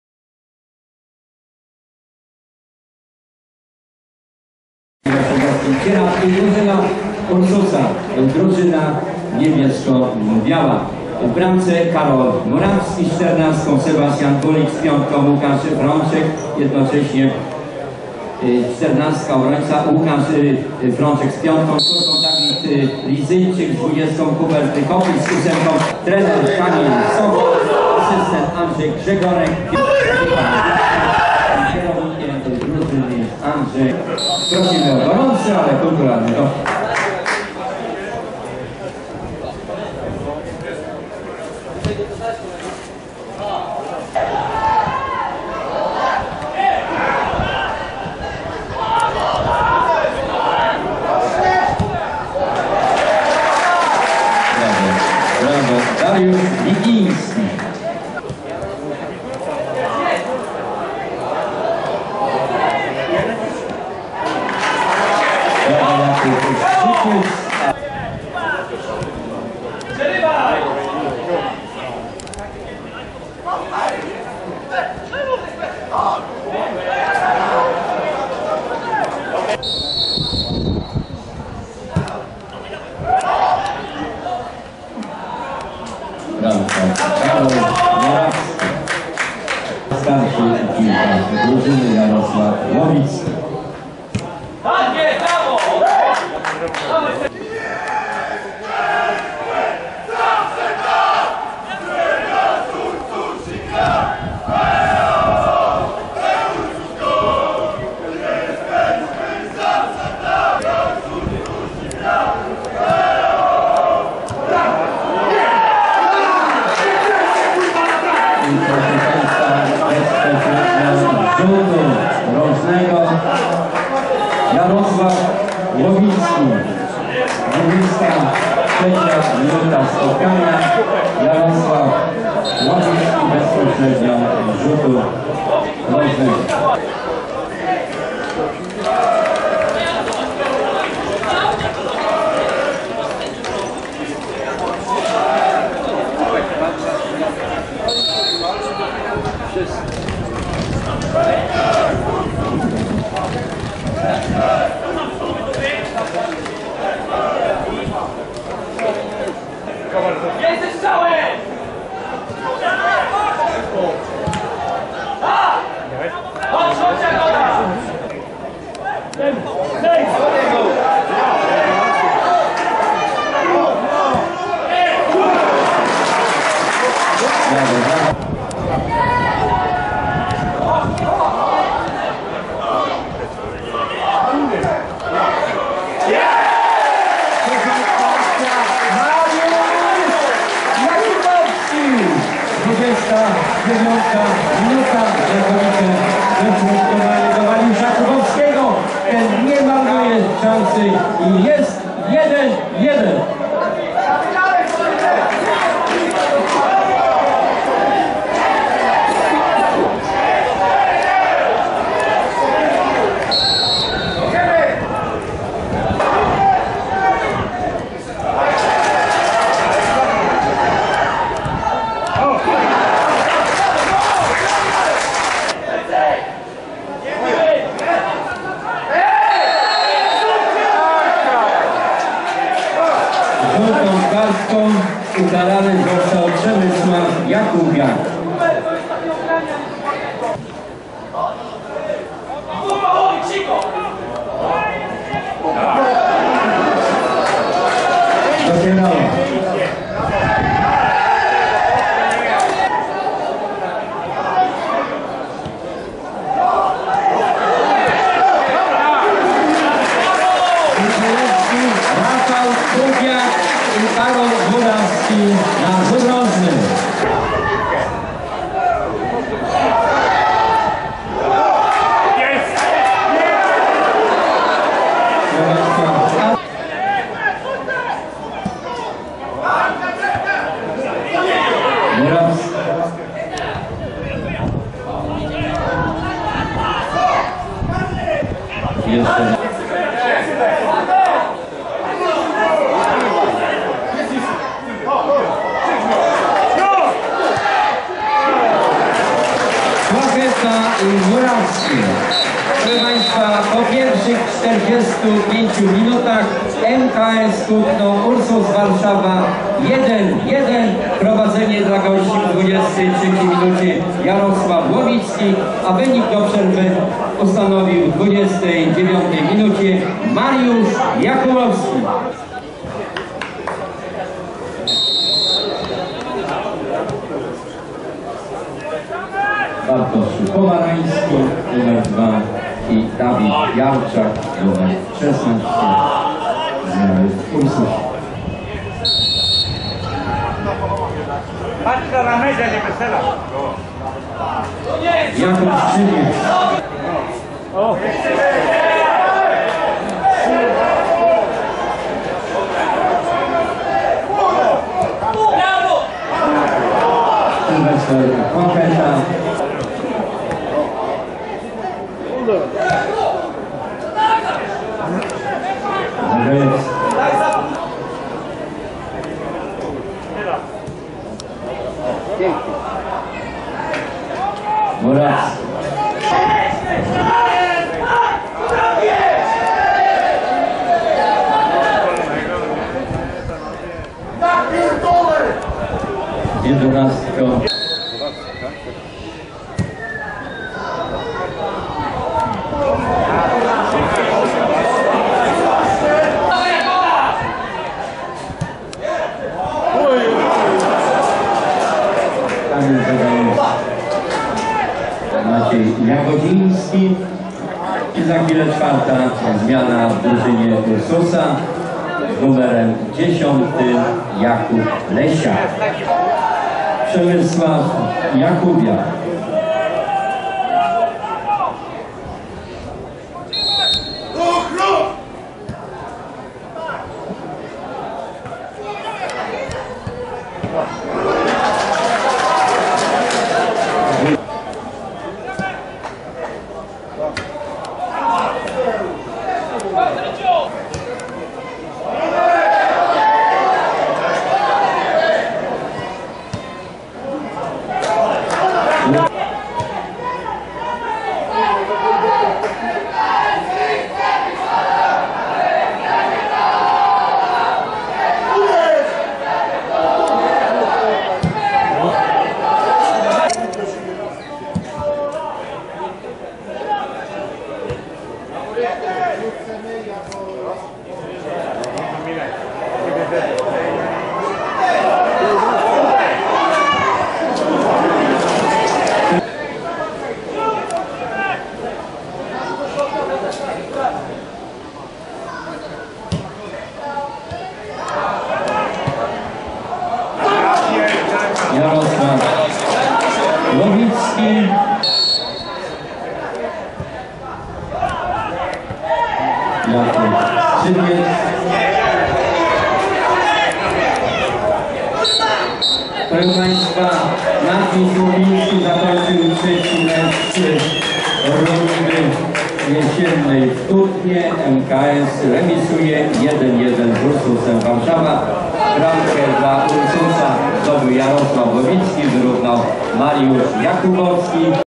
MKS Kutno - Ursus Warszawa 1:1 (1:1)
Skrót meczu 3. kolejki III ligi łódzko-mazowieckiej
video: Piotr Szcześniak - Miejscy.pl